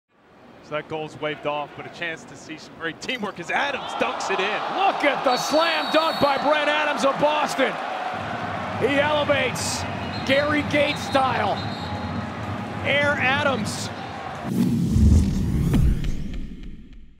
Brent Adams Elevates and Dunks Gary Gait Style
Boston Cannons All-Star Midfielder Brent Adams scores a memorable goal in the 2015 MLL All-Star game in Houston.